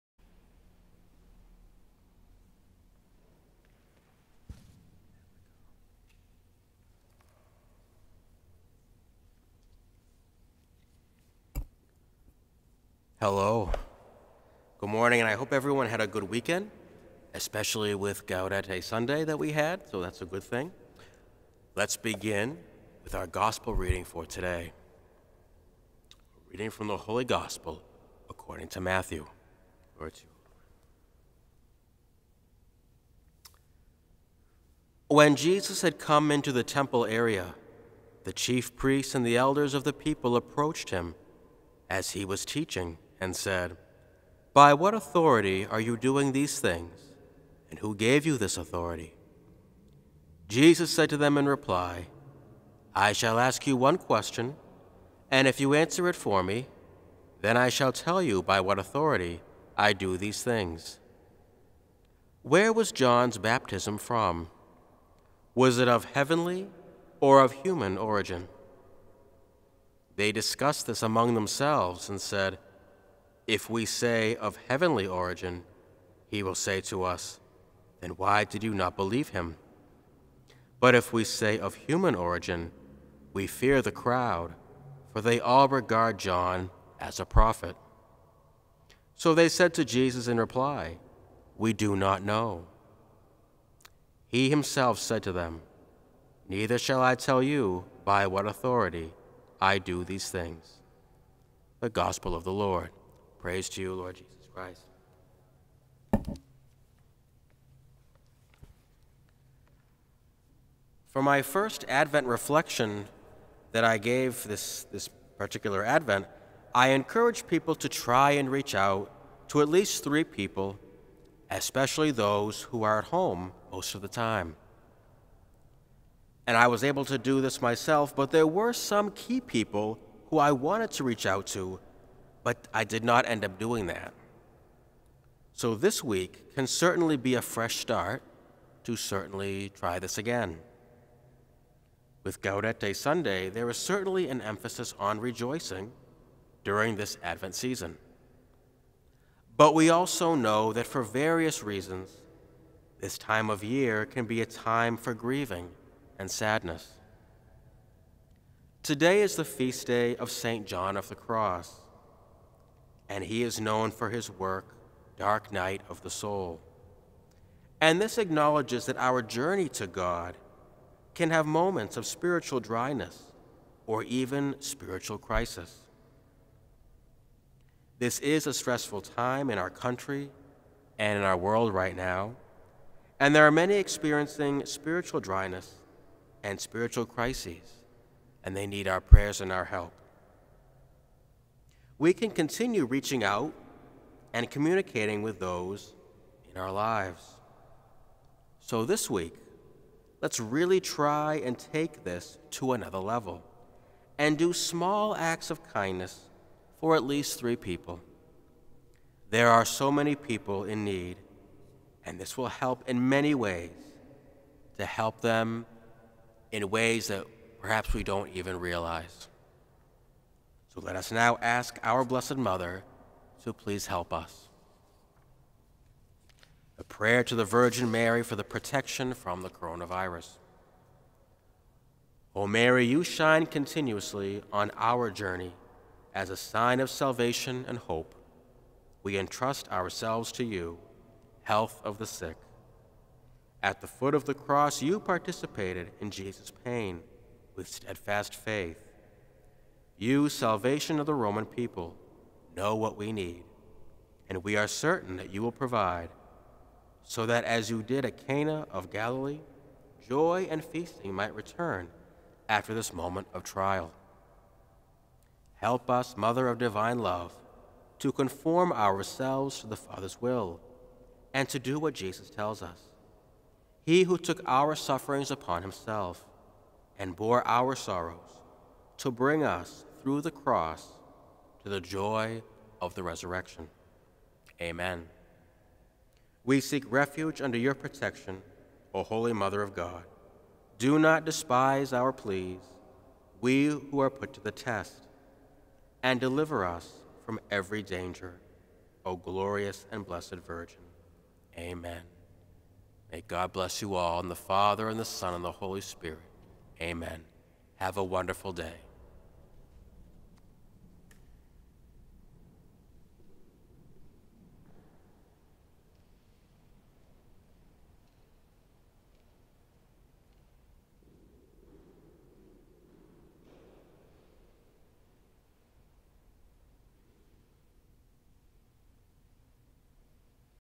Reflection for Monday of the Third Week in Advent
Fr. Kevin Upham, Parochial Vicar